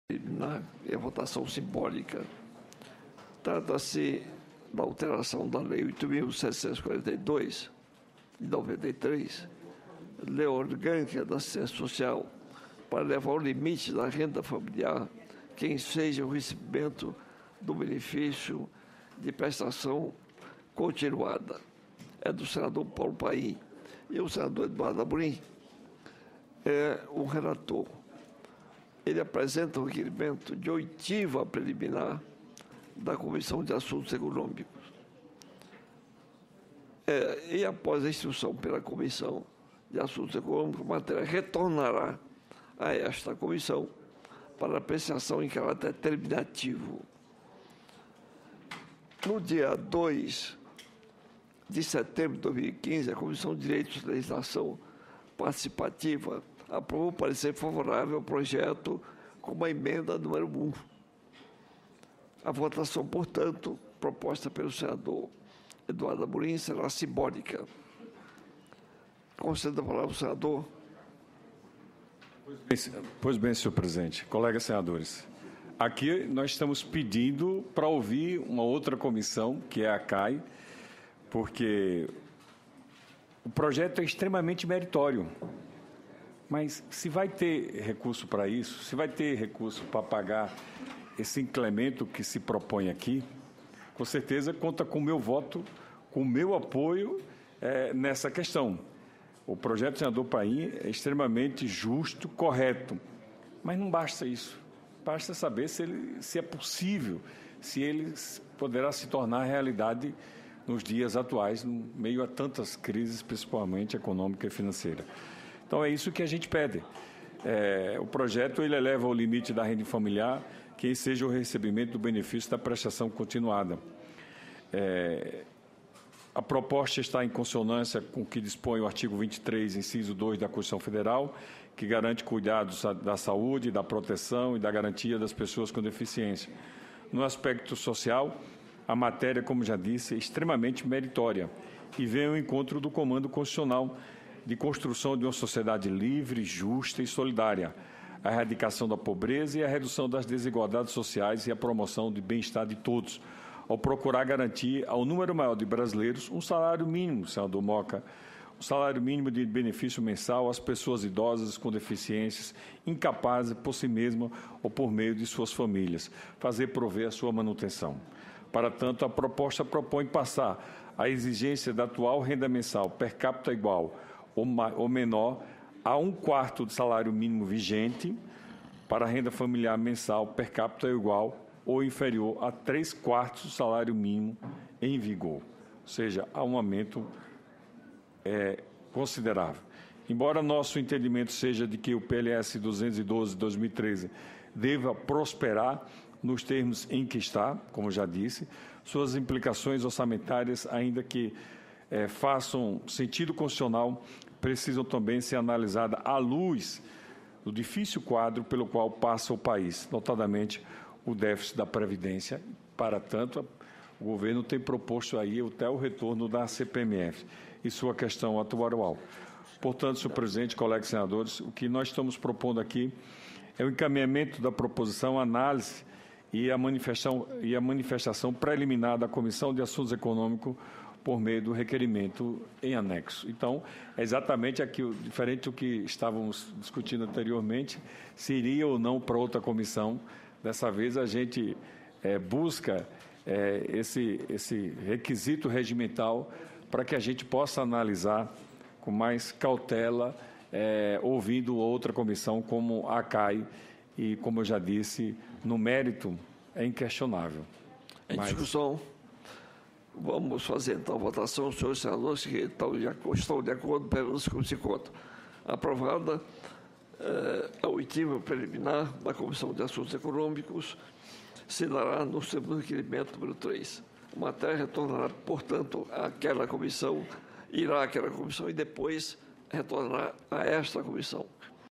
Aprovado projeto que eleva o limite da renda de quem recebe benefício de prestação continuada
02/03/2016 - CAS - Aprovado projeto que eleva o limite da renda de quem recebe benefício de prestação continuada